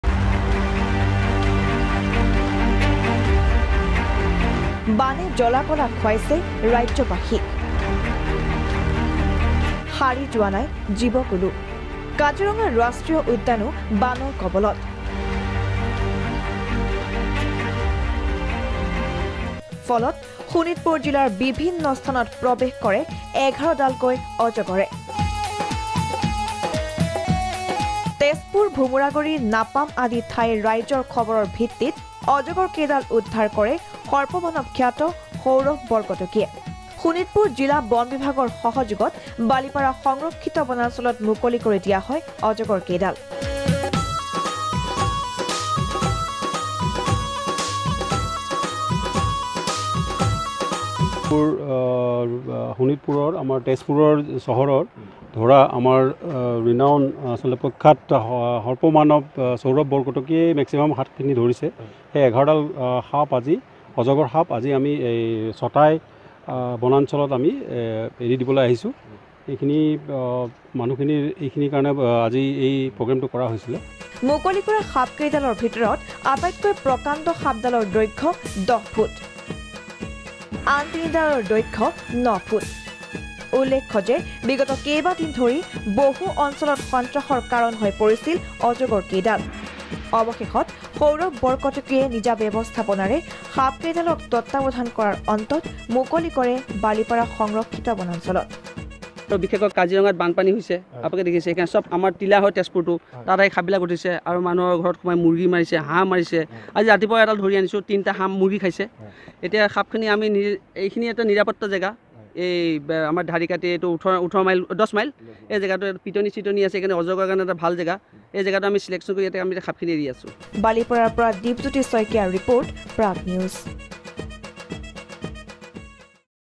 Pythons come out of holes in flood area of Balipara
Flood not only affects humans and their residential areas, but even plants and animals together. if humans lose their homes, so do animals lose their habitat. Flood is wreaking havoc everywhere. Pythons are coming out of their holes due to water getting inundated everywhere. At least 11 huge pythons were spotted wriggling out of their holes in Balipara's flood prone areas.